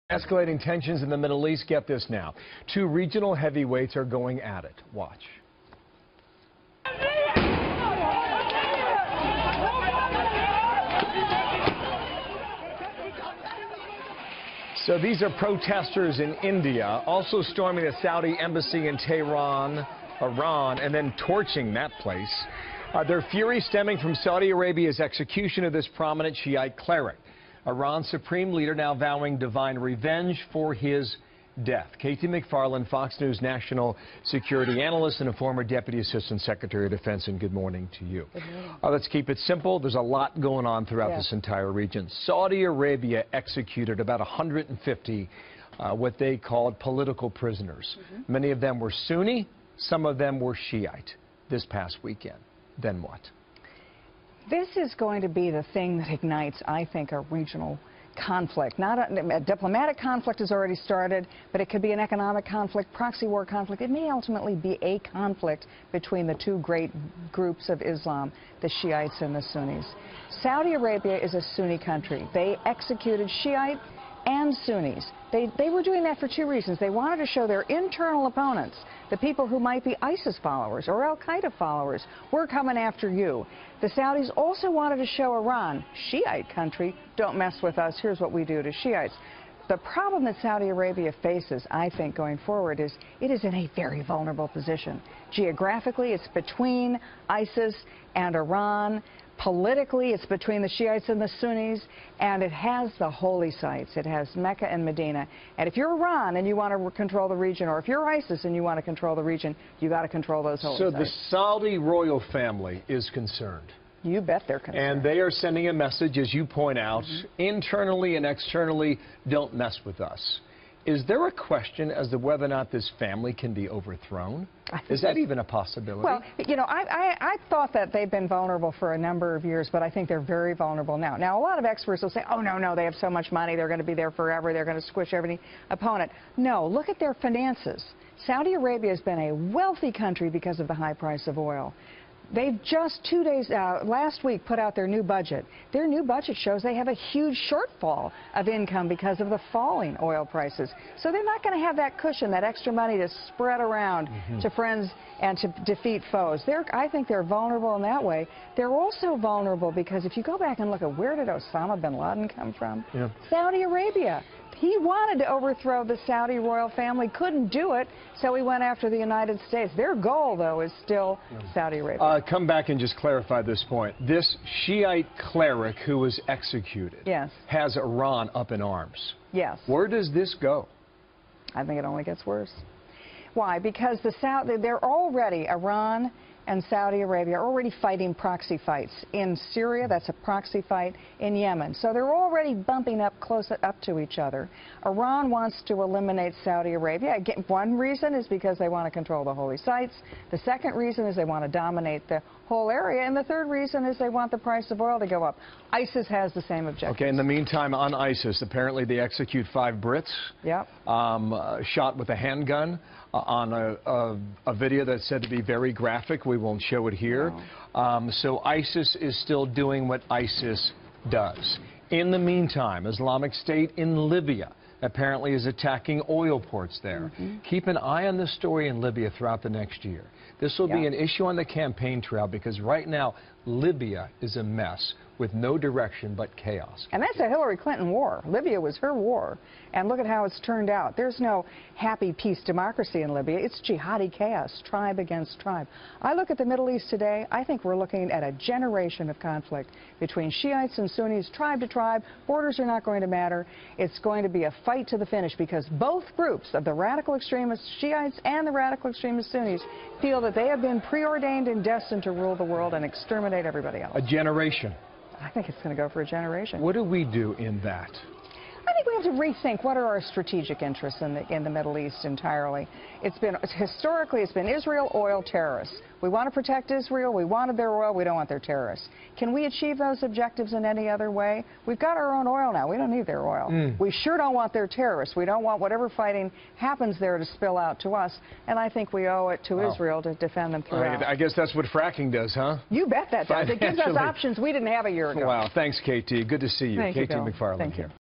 Iran-Saudi Arabia rift sparks fears of generational conflict
Fox News national security analyst KT McFarland explains fallout over execution of cleric